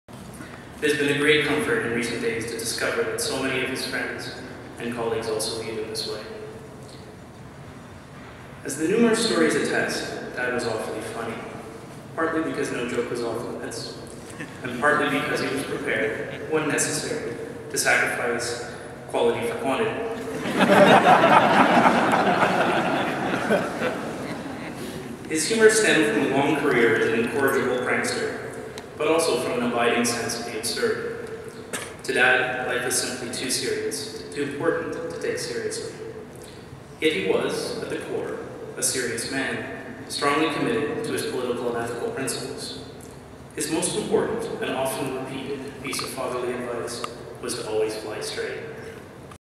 Jim Travers Memorial
Jim Travers's sons pay tribute to their father at his memorial serice on Tuesday night